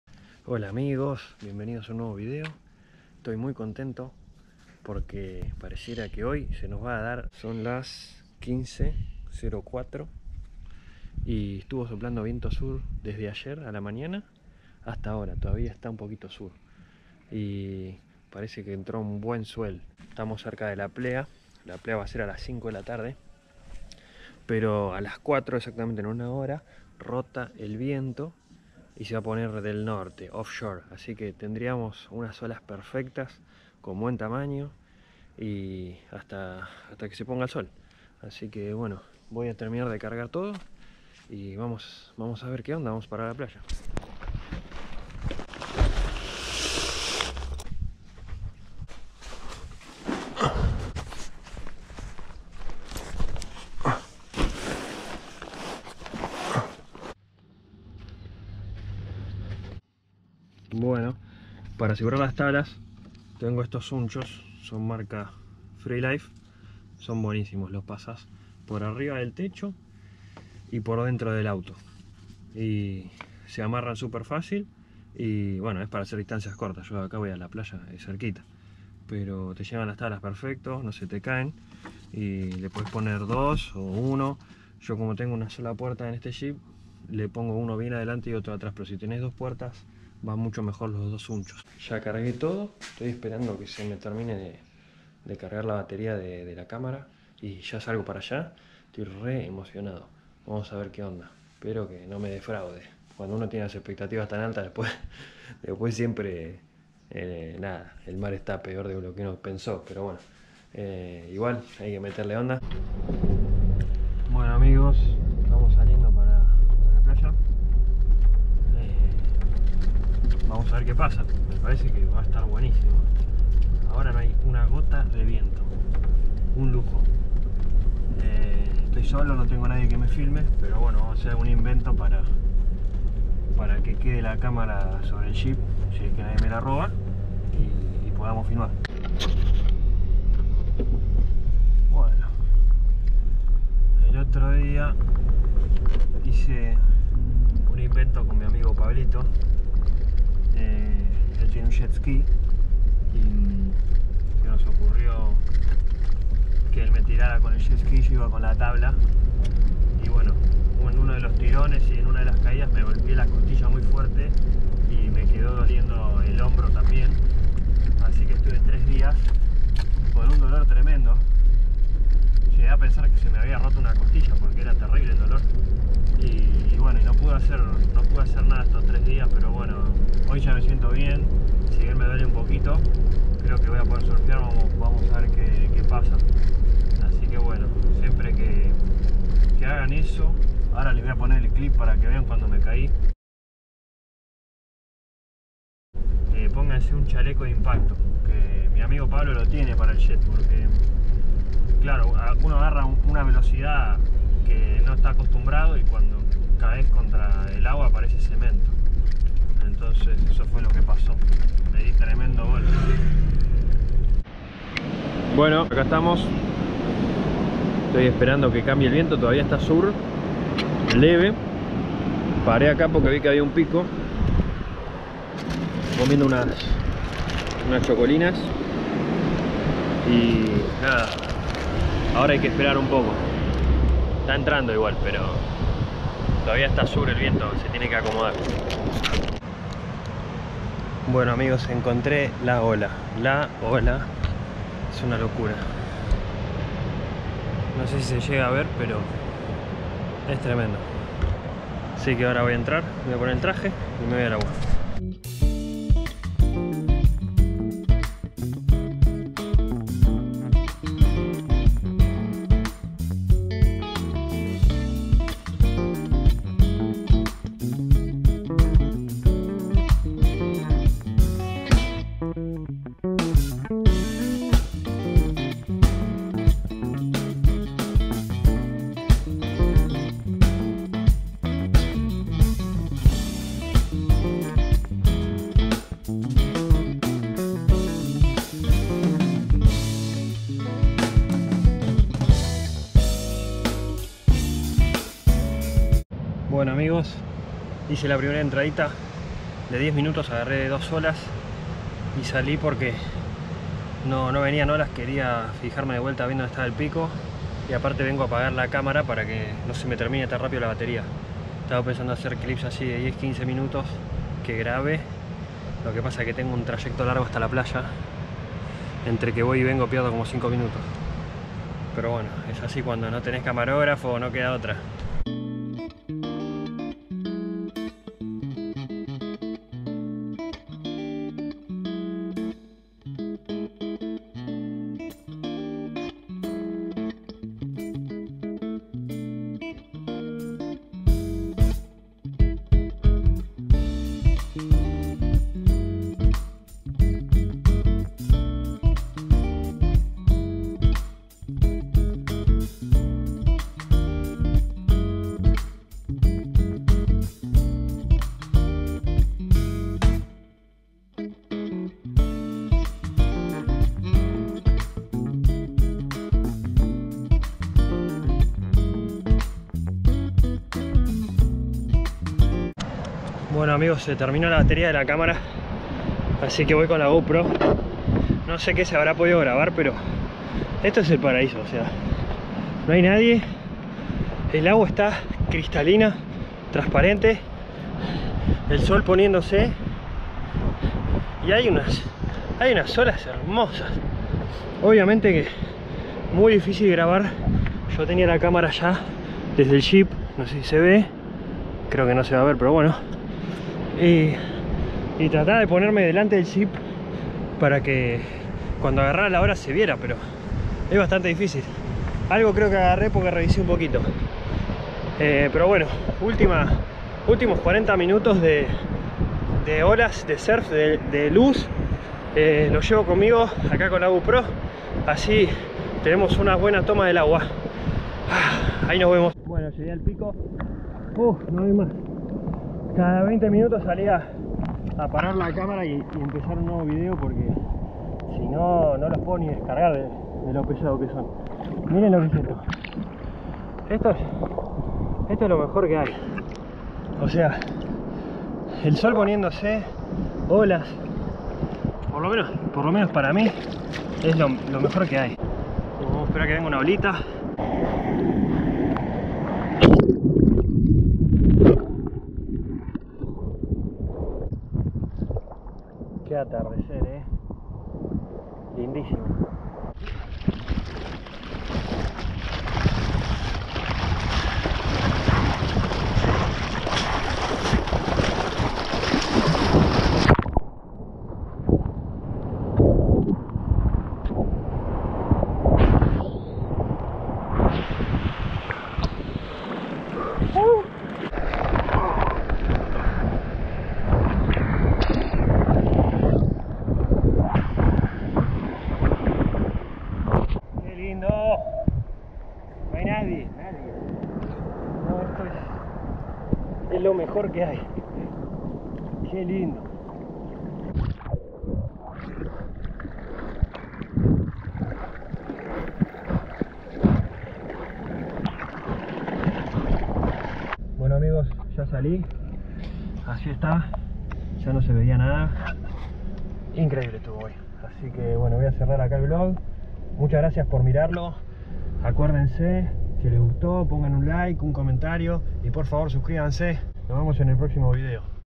Me fui a surfear en el JEEP
Busco una ola para surfear hasta el atardecer. Surf Vlog Argentina

Música por @leitancasot (Leandro Casot).